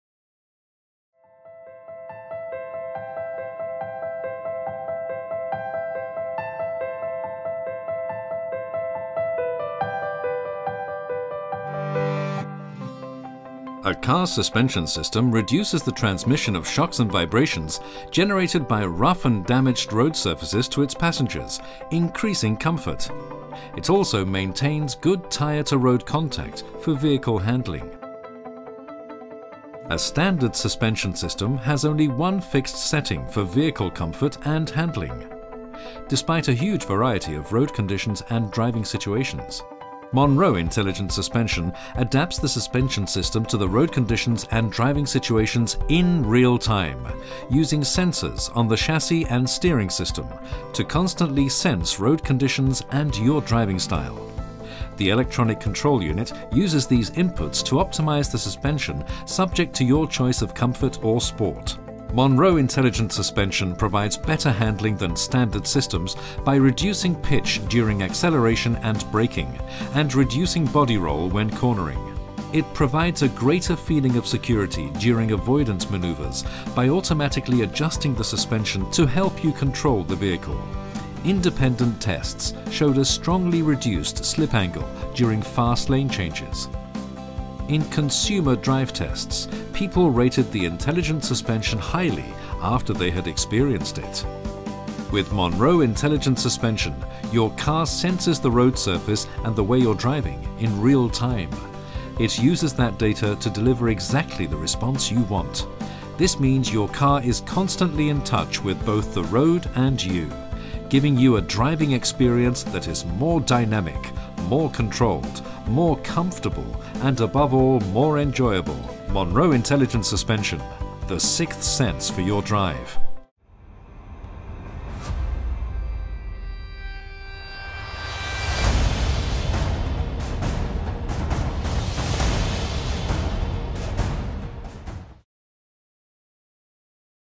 Monroe Intelligent Suspension
A standard suspension system has only one fixed setting for vehicle comfort and handling, despite a huge variety of road conditions and driving situations. Monroe Intelligent Suspension adapts the suspension system to the road conditions and driving situation in real time using sensors on the chassis and steering system.